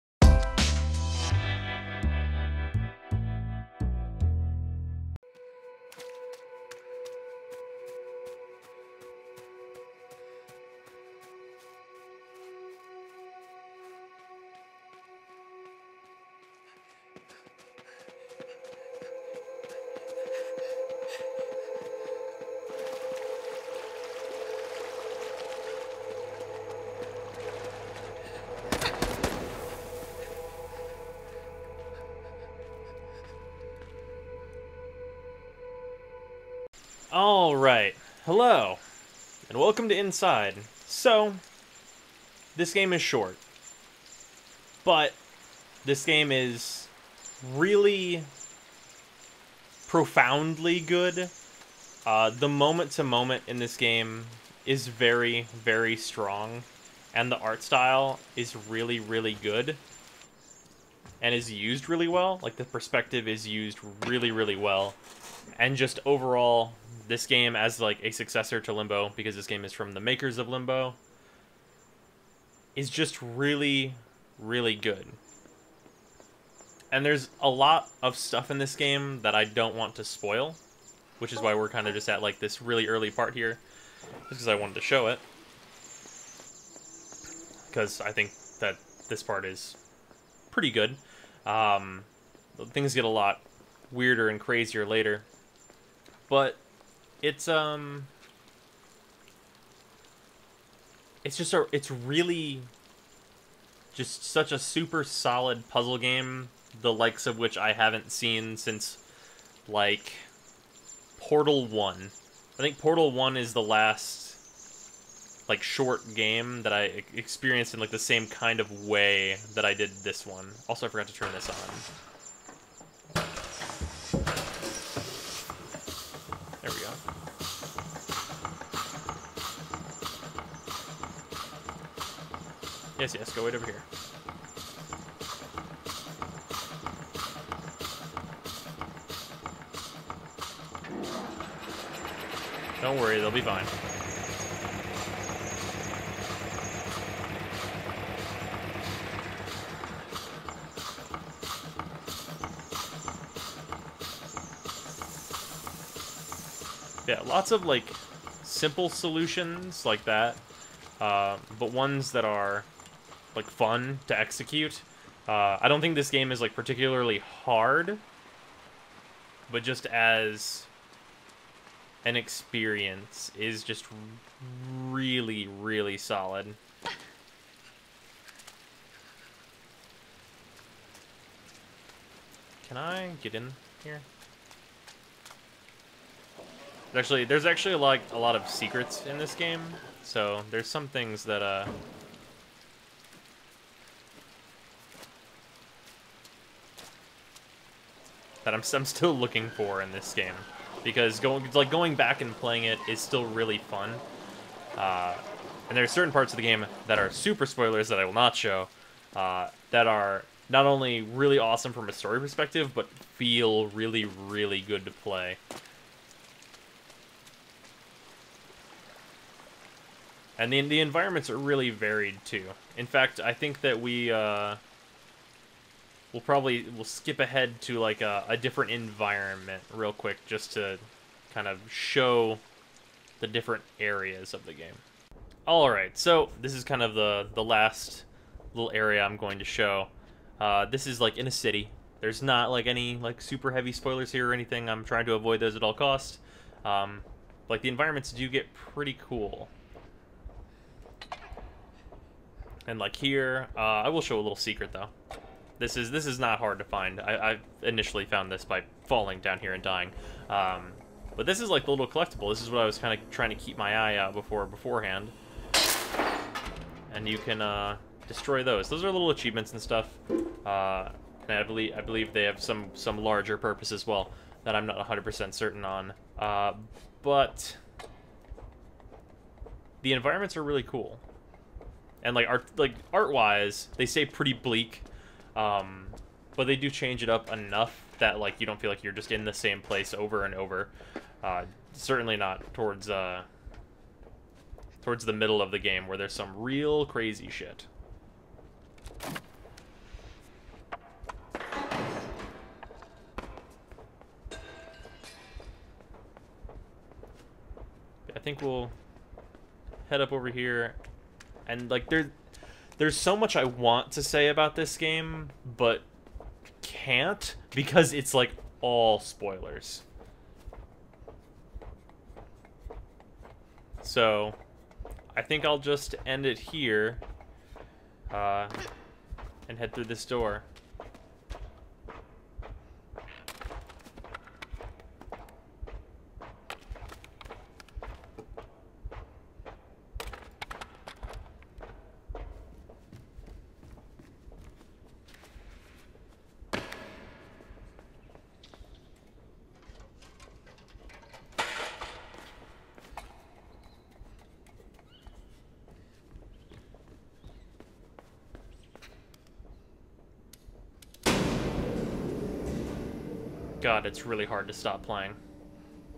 Why Would You Play #4: INSIDE
It's really hard to not spoil this game and still say what's so good about it.

V Follow me on the Socials V